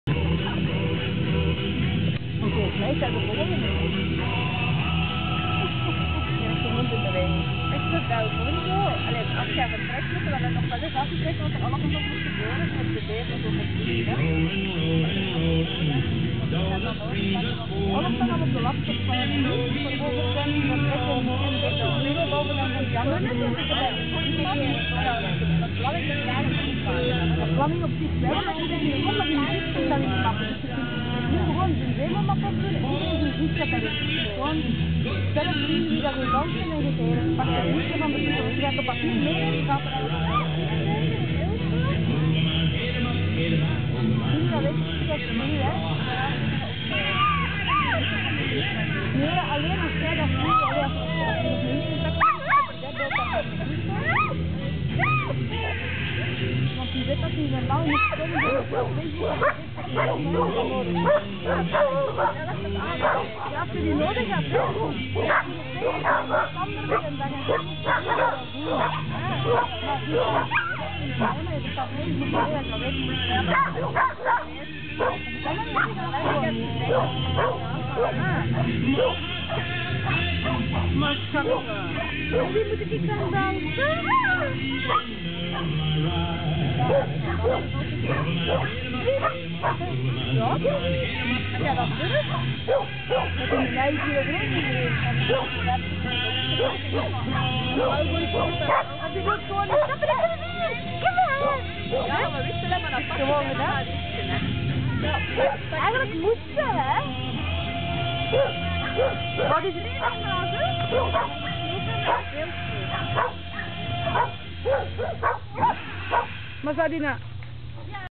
Demo Dogdance De Schakel - 14 juni 2015 - Demo 1 - Lutgarde en Sam
Demo doggydance met onze hondjes op de opendeurdag van dierenasiel De Schakel in Tielt-winge, op weer een heel zonnige en warme zondag, 14 juni 2015 - Lutgarde en Sam